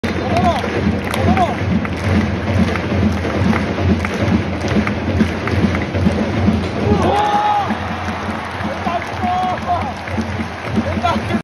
アルビレックス新潟！GK小島、矢野貴章のPKを魂のストップ！
Ｊ2リーグ第22節　アルビレックス新潟VS栃木SC 84分　矢野貴章のPKを小島亨介がストップ！